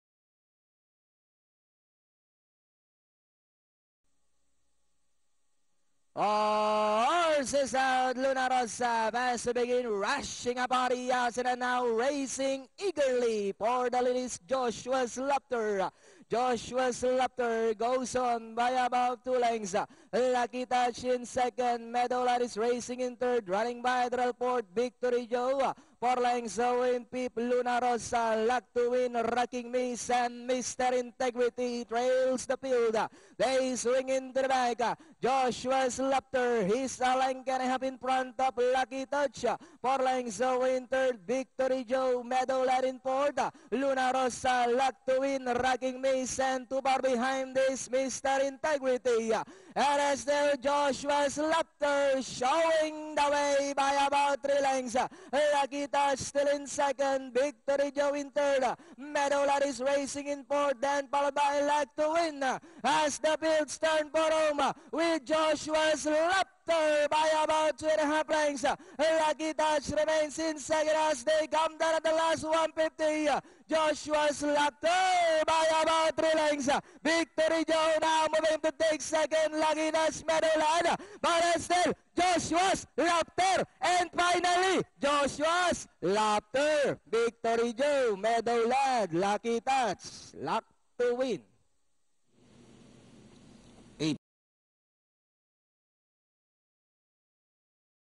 MHP SLLP JULY 3, 2010 RACE 8 JOSHUA'S LAUGHTER.mpg
RACE RESULT: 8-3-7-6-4  TIME: 1:21.6  RIDDEN BY: MA ALVAREZ